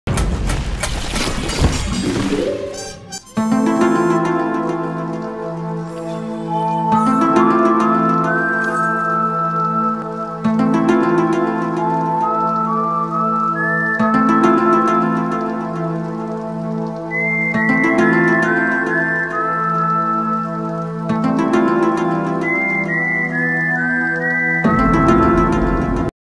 The Potato Files
I found this funny glitch and made a video on it